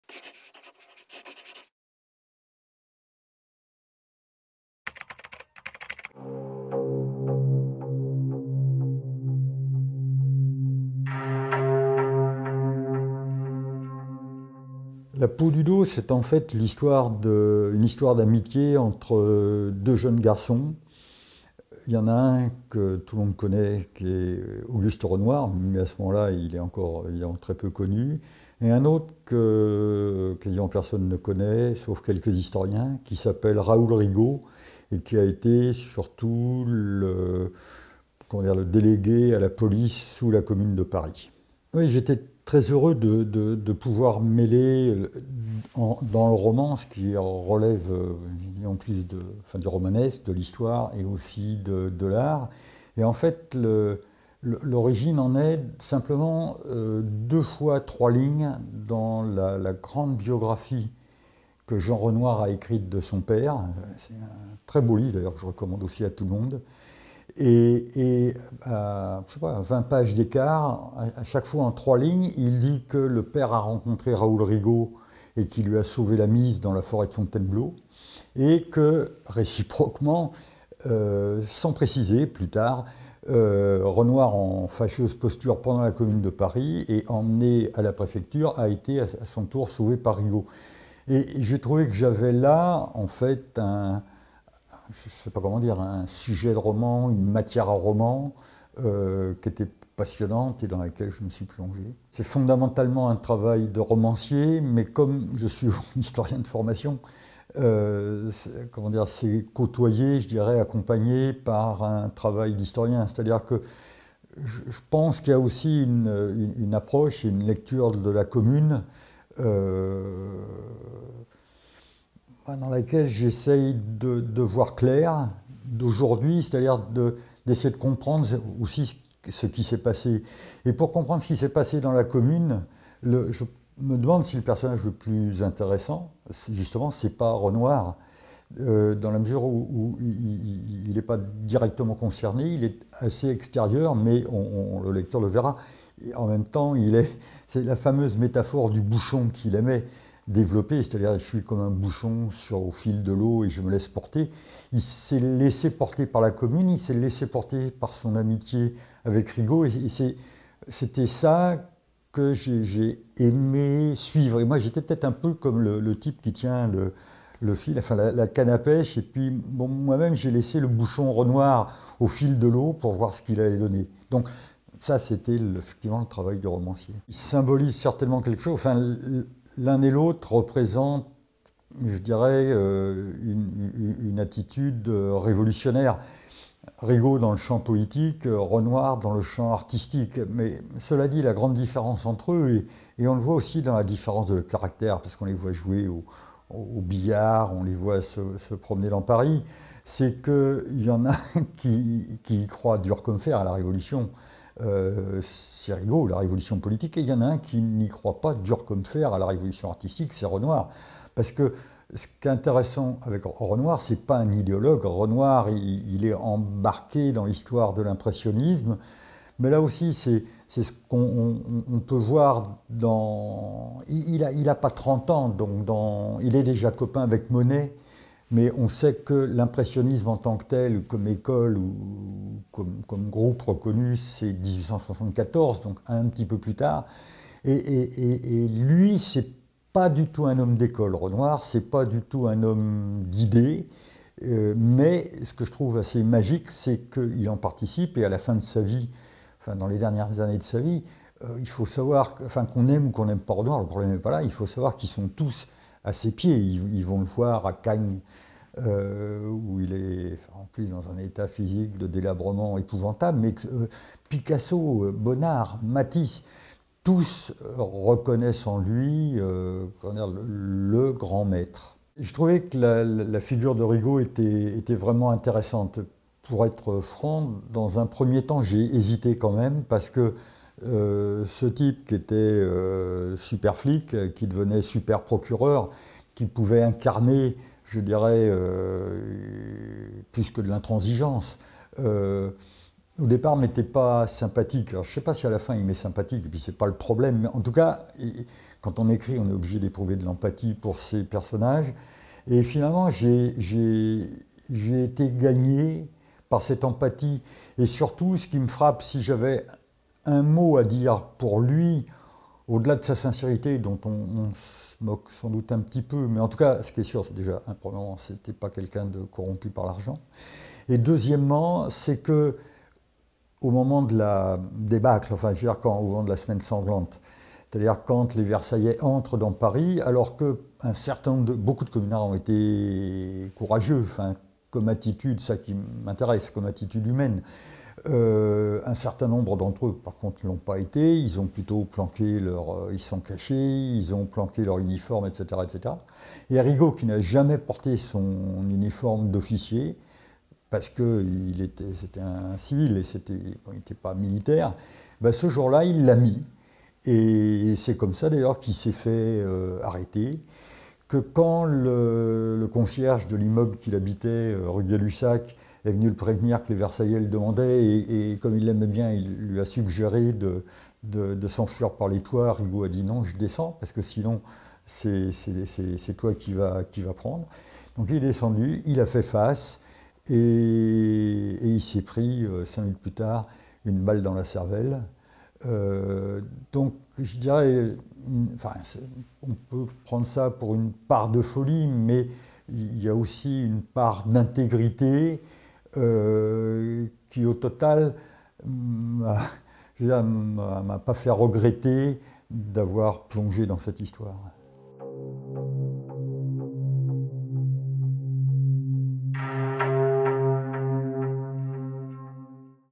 Bernard Chambaz - La peau du dos
A l'occasion du Festival de Nancy "Le livre sur la place" 2022, Bernard Chambaz vous présente son ouvrage "La peau du dos" aux éditions du sous-sol. Rentrée littéraire automne 2022.